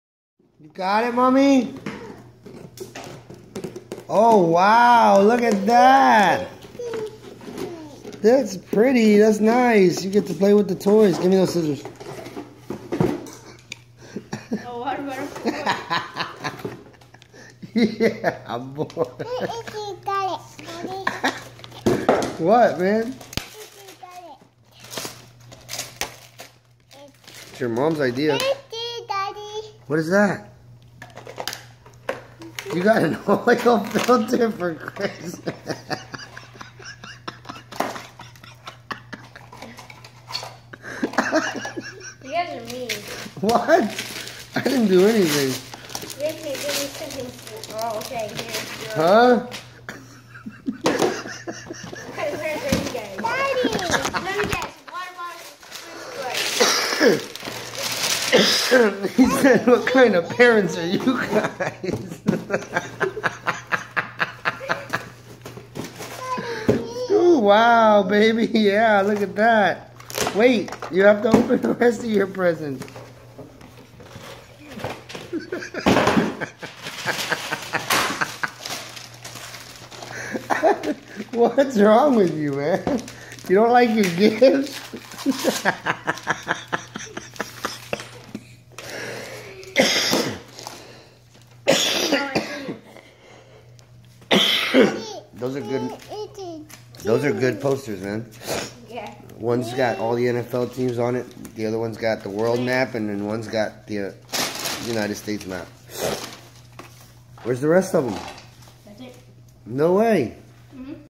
Bad kid fake presents
Edgar opening his presents for xmas